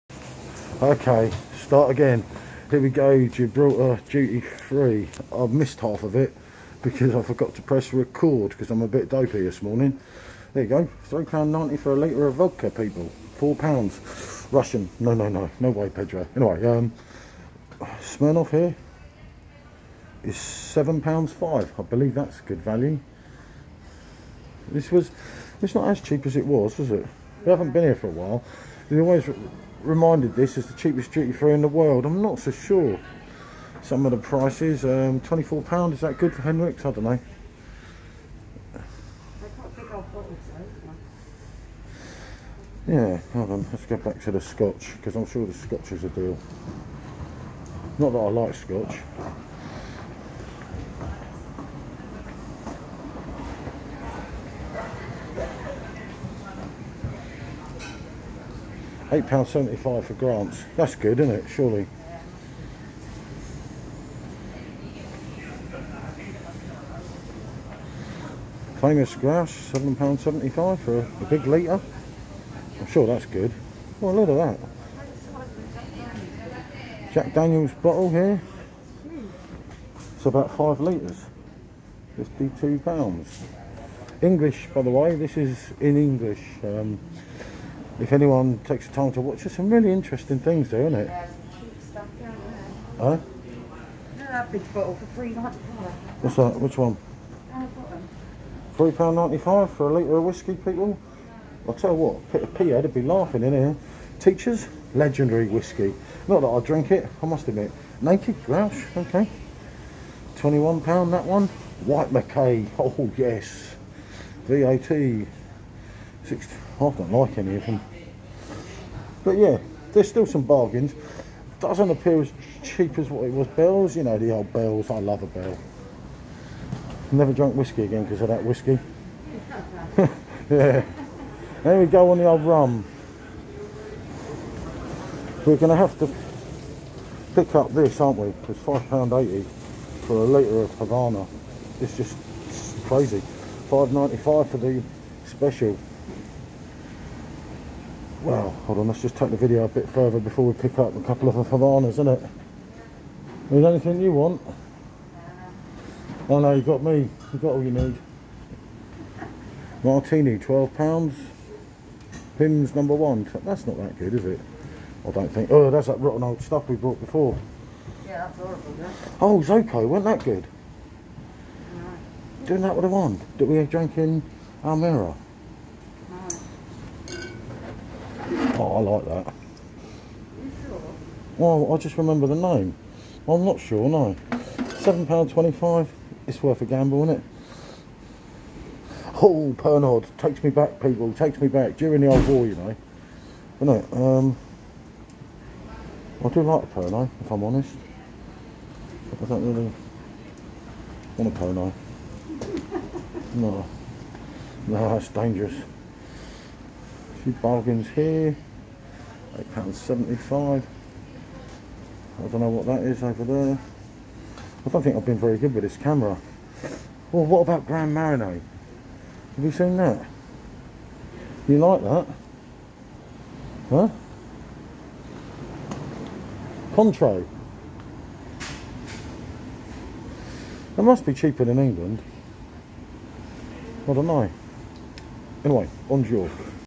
Gibraltar Duty free look at some of them prices cheapest in world 🌎 ?
Wow walked out with 5 litres of sprits Michelle said why you buy u don't like them I say because its cheap LOL 🙂